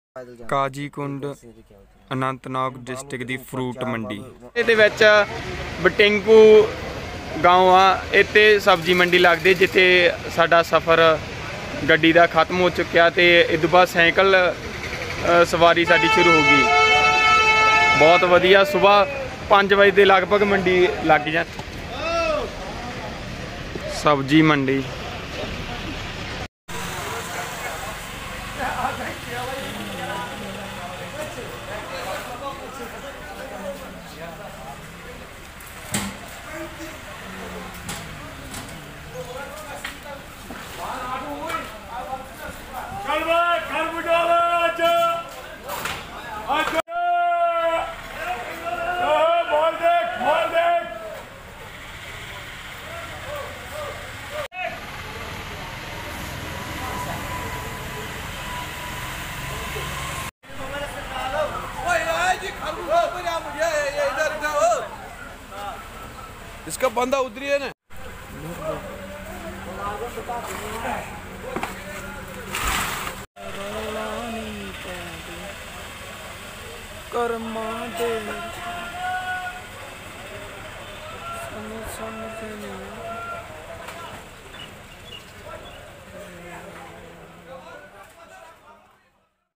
Kanjigund Anantnag Mandi ਦਾ ਦ੍ਰਿਸ਼ (ਝਲਕ) ਇਕ ਨਜ਼ਰ ਸ਼੍ਰੀ ਨਗਰ ਦੀਆਂ ਮੰਡੀਆਂ ਵੱਲ
leh Ladakh tour ਦੇ ਰਾਹ ਚ batango ਪਿੰਡ ਦੀ ਮੰਡੀ